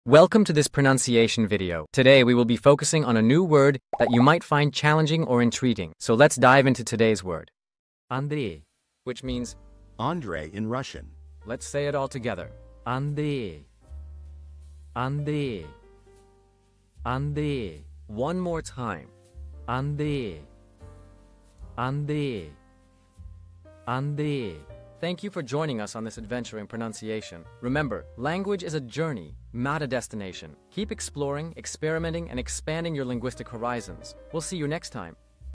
How to pronounce Andrej | Андрей (Andrey in Russian)
Master the Pronunciation of 'Andrej | Андрей' - which means : Andrey in Russian 📝 with @PronunciationRussian 🎓🗣️ - [Your Guide to Russian]

Learn to pronounce 'Andrej' perfectly in Russian with our easy and engaging tutorial by @PronunciationRussian! 📖📽️ In this comprehensive video, we provide you with native speaker examples 🗣️👂 and phonetic breakdowns 🎵 to ensure you get the pronunciation just right.